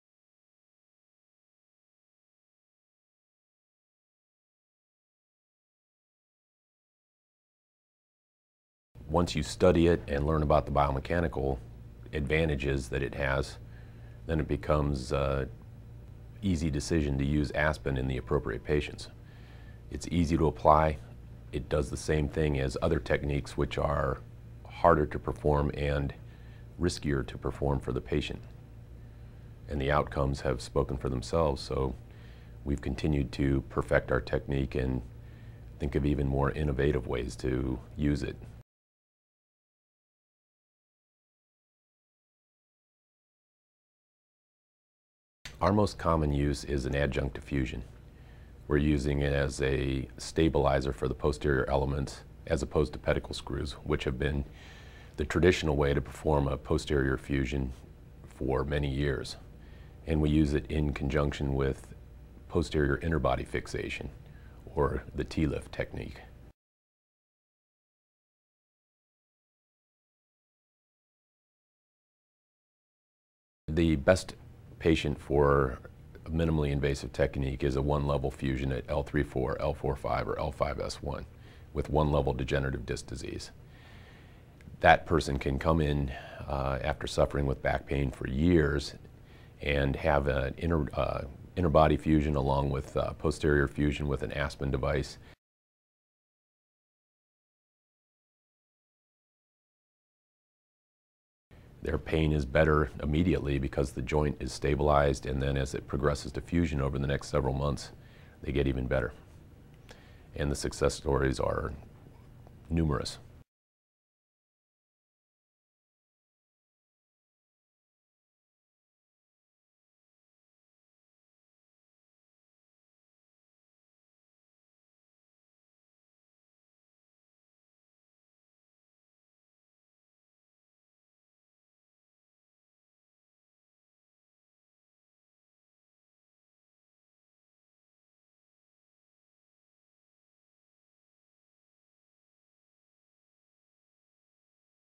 Neurosurgeon Dr. Glenn Keiper: ASPEN® MIS Fusion System
Dr. Glenn Keiper of the Spine Surgery Center of Eugene in Oregon explains the advantages to using the ASPEN device in the appropriate patients and why he has adopted the system in his practice.